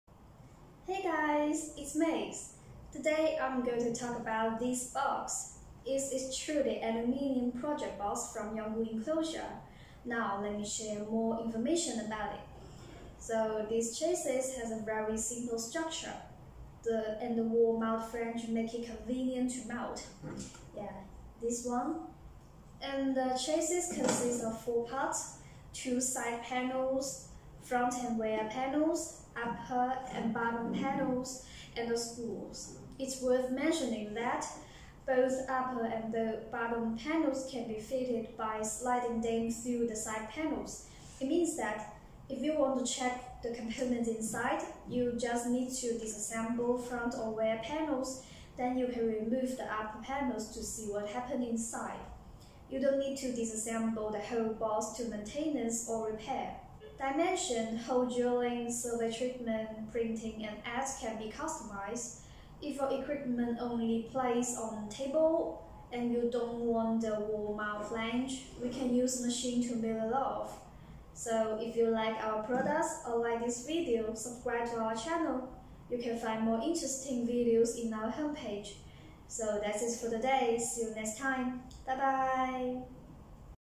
Alu chassis without limitation.
This is a chassis that can customize dimensions as you wish, and it has very simple structure, therefore it's easy to install and assemble.